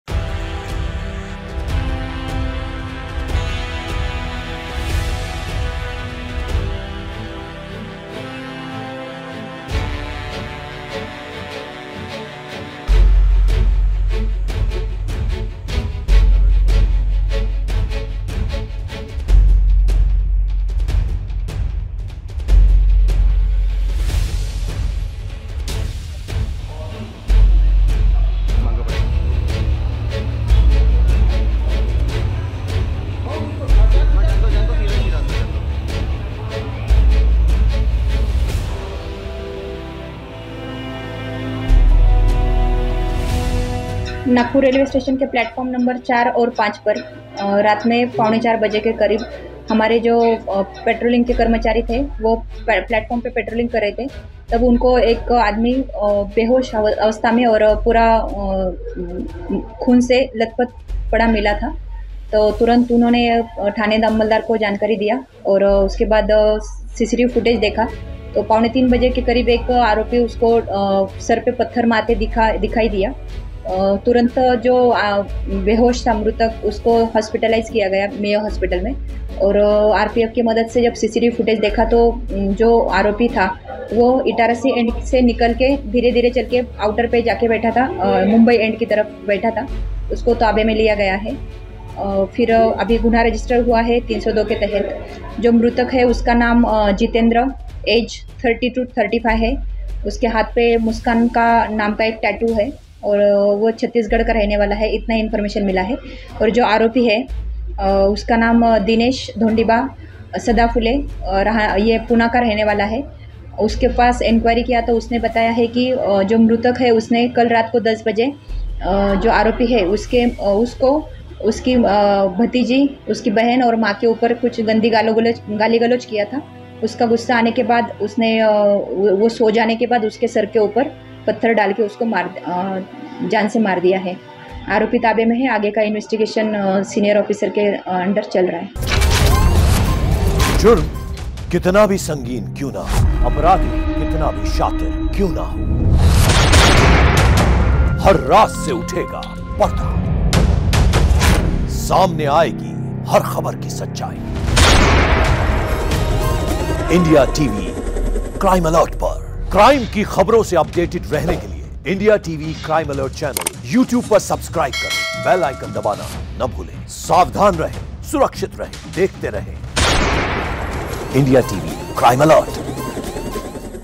शख्स को मां- बहन की गाली देना पड़ा भारी! Nagpur का अचंभित कर देने वाला मामला | IndiaTV
नागपुर का रेलवे स्टेशन अत्यधिक संवेदनशील माना जाता है ,लेकिन यहाँ एक हत्या का मामला सामने आया है। जहां एक व्यक्ति ने ,एक दूसरे व्यक्ति की पत्थर से कुचलकर निर्मम हत्या कर दी। यह घटना प्लेटफार्म नंबर 5 पर देर रात 2:40 बजे के दरमियान की है। मृतक की पहचान छत्तीसगढ़ निवासी जितेंद्र उर्फ टोपी के रूप में हुई है। जबकि आरोपी दिनसागर उर्फ दिनेश धोंडिबा सदाफुले महाराष्ट्र के पुणे का रहने वाला है, लोहमार्ग पुलिस ने सीसीटीवी की मदद से आरोपी को गिरफ्तार कर लिया है।

Nagpur's railway station is considered very sensitive, but a murder case has come to light here. Where a person brutally murdered another person by crushing him with a stone. The incident took place on platform number 5 around 2:40 am. Jitendra has been identified as Jitendra Topi, a resident of Chhattisgarh. While ten days Ayarsagar Dinesh Dhondiba Sadaphule is the fifth occupant of Maharashtra, Lohmarg police has arrested the crime with the help of investigation.